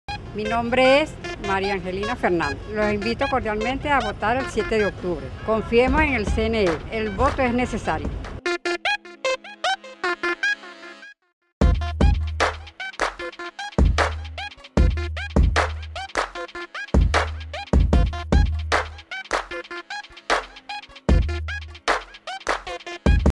VOTAResMANDARINA: El voto es necesario
LaraTvec nos presenta sus micros "Votar es Mandarina", llamado a participar en el proceso electoral del 7O

LaraTvec, Televisora Comunal de Barquisimeto, ha registrado múltiples voces llamando a votar el 7 de Octubre, expresiones ciudadanas que reafirman la democracia participativa y protagónica, el empeño de un pueblo de reafirmar los cambios asumiendo todos los mecanismos que la Constitución Bolivariana entrega como vías de consolidación del sueño de Bolívar.

Entre ellas, el día del simulacro, una señora expresa su opinión sobre lo que será la elección: "Es más fácil que pelar una papa, pelar una mandarina", palabras que sintetizan el minuto que estaremos frente a la máquina electoral o el Sistema de Autenticación Integrado, soporte tecnológico que nos sitúa entre los más avanzandos del mundo, con auditorías realizadas y por realizar en cada una de sus etapas de funcionamiento.

Así dos ideas se mantienen presente en las mayorías de los testimonios: La facilidad de votar y el total respaldo al Consejo Nacional Electoral, una de las instituciones de mayor credibilidad hoy por hoy en el país.